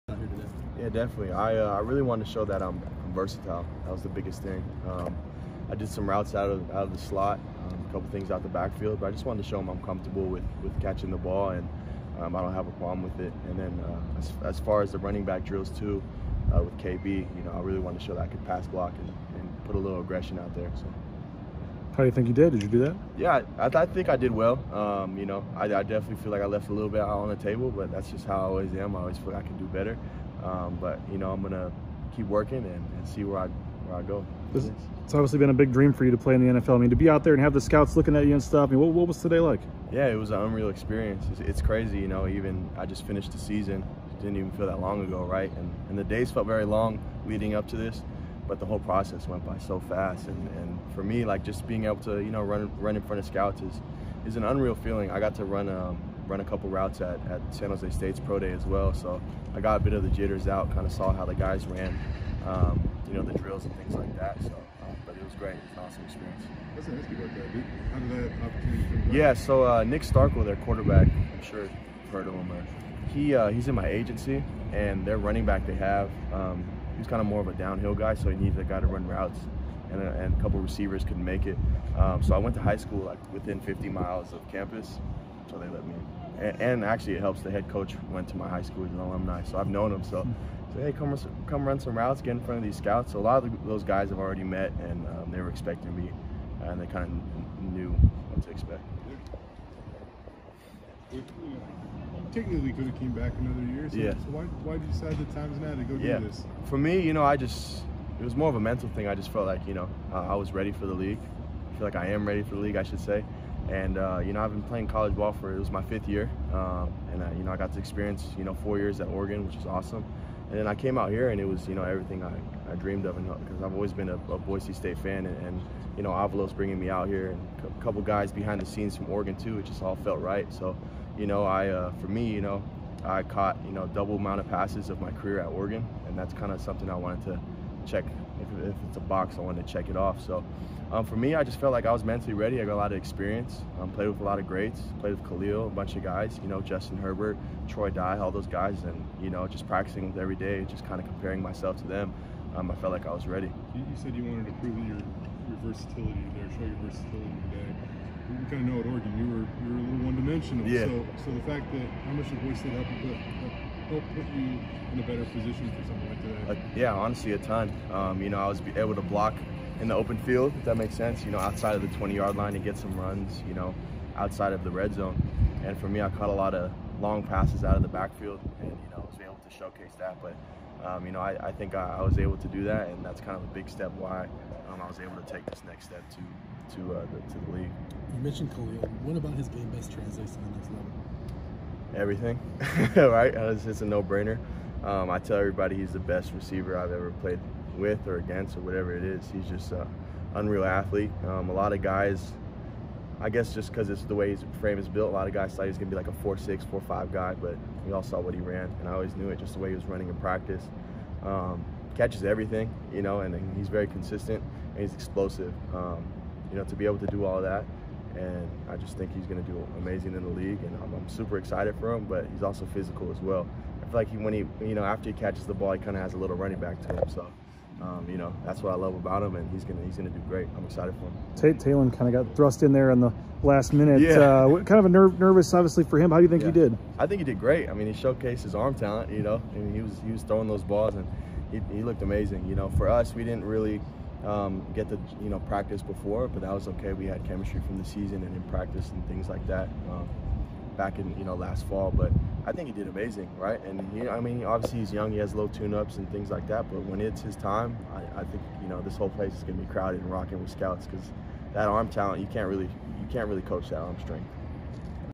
Cyrus Habibi-Likio talks after working out at Boise State Pro Day (3-30-22)
Former Boise State running back Cyrus Habibi-Likio talks with the media after working out at Pro Day on Wednesday, March 30th, 2022. (Video by B.J. Rains/Bronco Nation News)